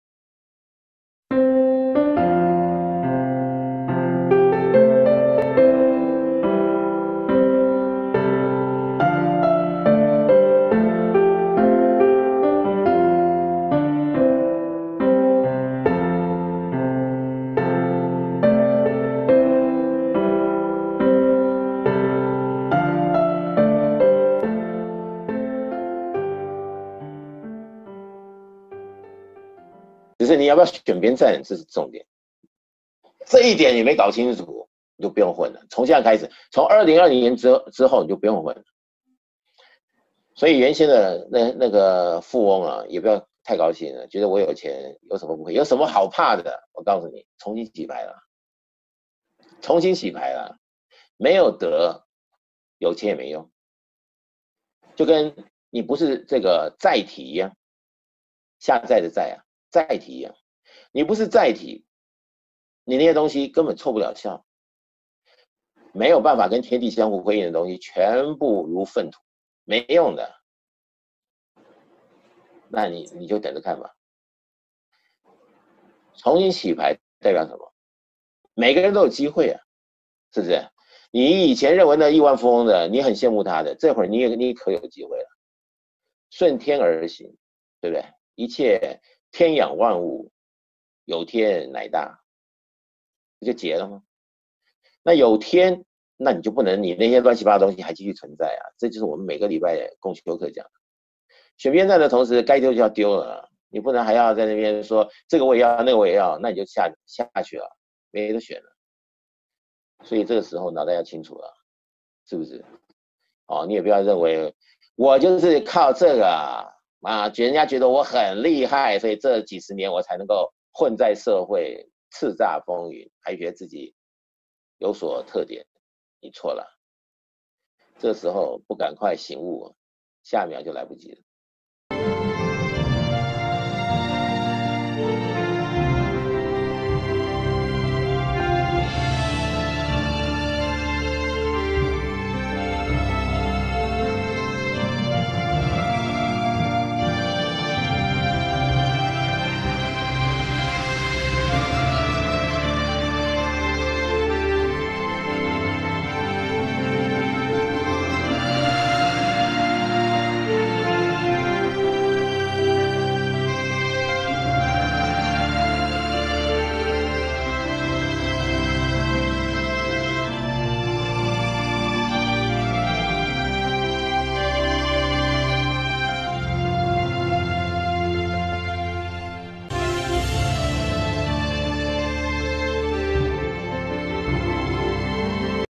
按天理而行｜太陽盛德導師–超級生命密碼網路共修
選邊站、按照天理而行，有德成為載體後，才能擁有自己的一片天。

🙋我想參與網路共修🙋
👉步驟一：
(🔔提醒務必於天圓文化官網先註冊Email後才開始填單）

👉步驟二：
💚3. 網路填單購買